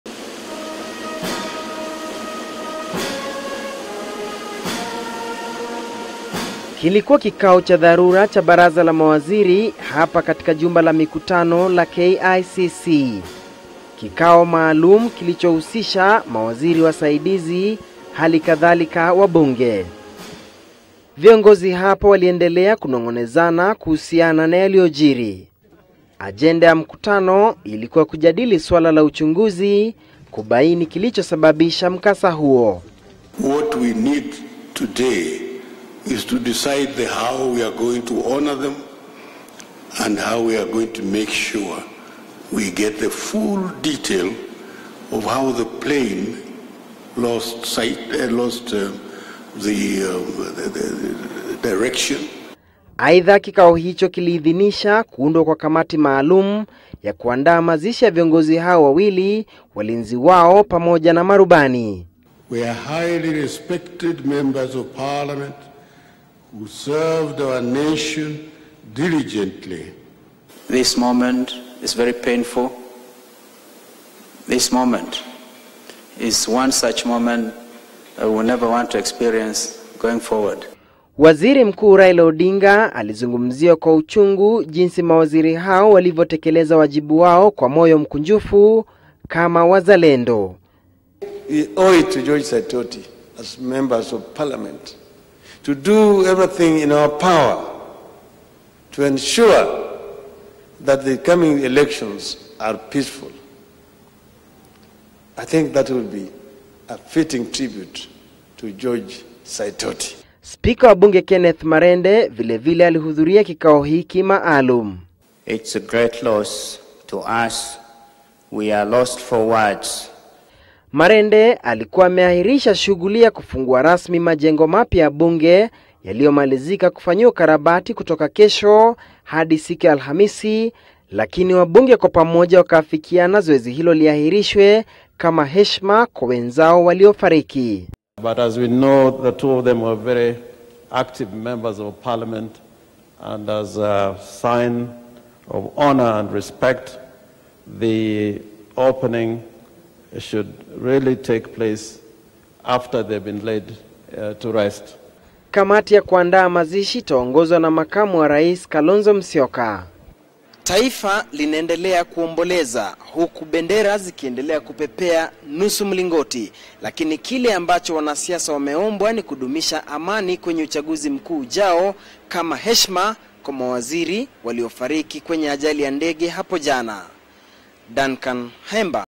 Ajali ya Helikopta
Tume maalum kubuniwa kubaini ajali ya helikopta iliyowaua Saitoti na Ojode.